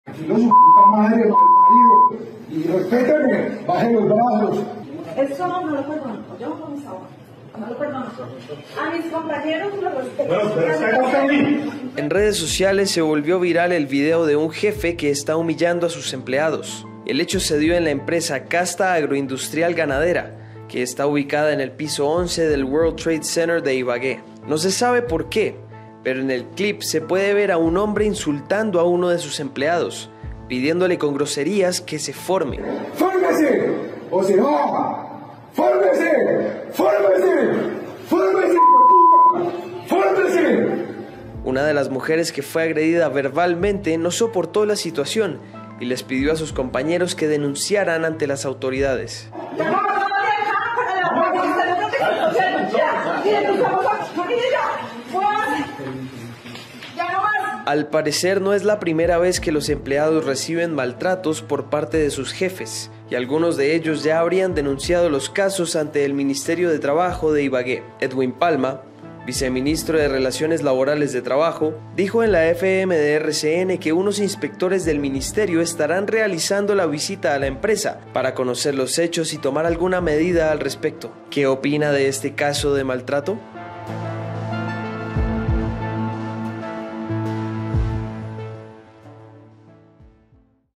Caso de maltrato laboral en empresa de Ibagué
Se conoció un video en el que los jefes de una empresa en Ibagué agreden verbalmente a sus empleados. Acá el video.

¿Qué opina de este caso de maltrato?